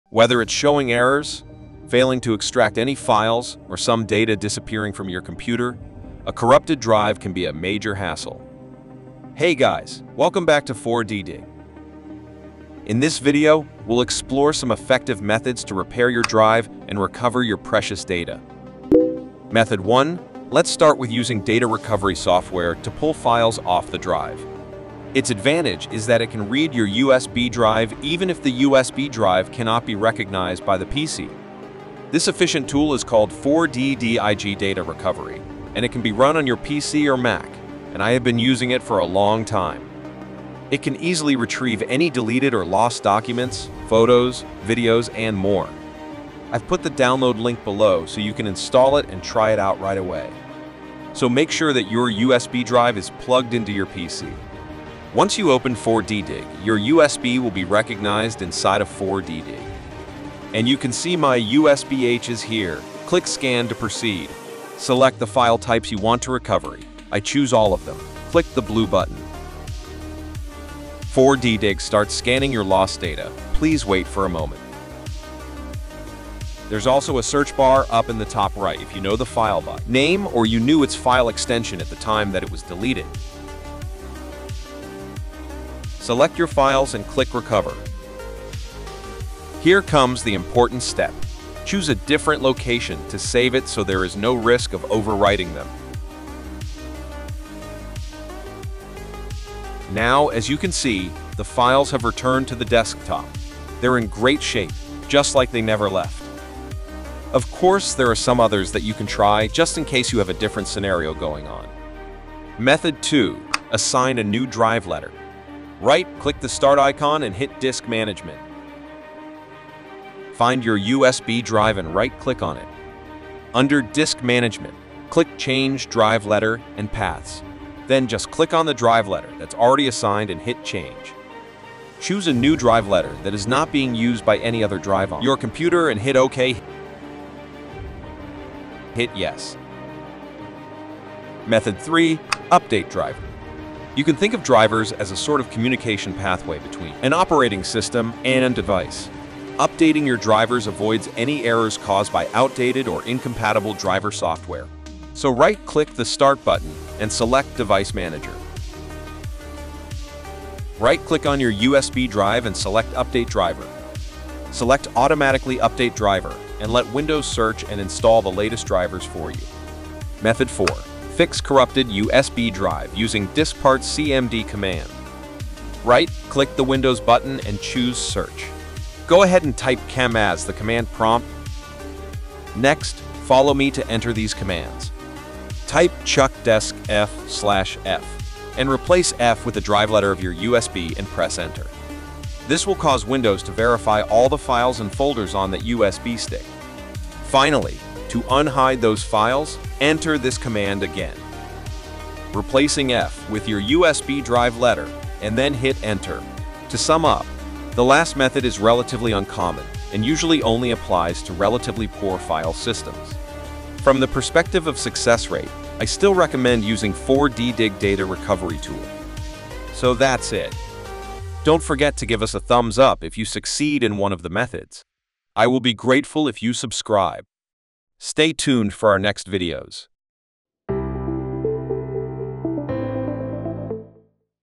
[FULL] How to Repair Corrupted USB Drive and Recover Data
In this video, we'll explore some effective methods to repair your drive and recover your precious data.

📌Timestamps:
00:00 Intro
00:20 Method 1: Recover Data from A Corrupted USB Drive with 4DDiG
01:53 Method 2: Assign a New Drive Letter
02:22 Method 3: Update Driver
02:51 Method 4: Fix Corrupted USB Drive Using Diskpart CMD Commands